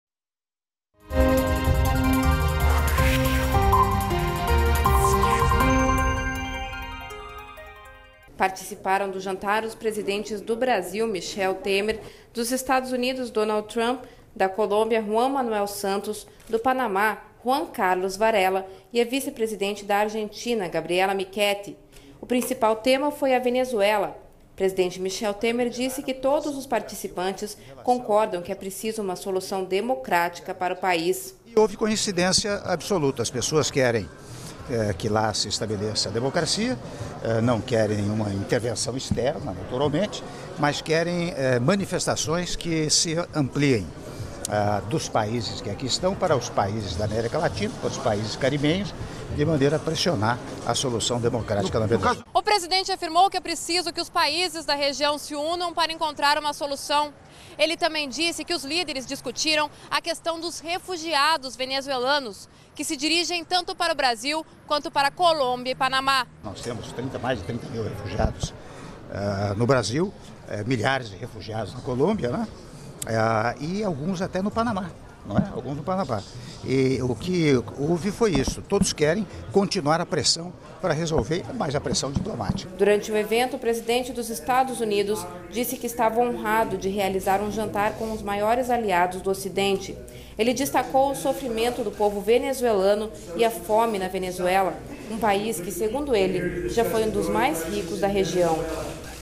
Donald Trump recebe Michel Temer durante jantar em Nova Iorque
REPÓRTER NBR - 19.09.17: Presidente Michel Temer chega à Nova Iorque para participar da Assembleia Geral das Nações Unidas. Temer foi recebido em um jantar pelo presidente americano Donald Trump, onde o assunto principal foi a Venezuela.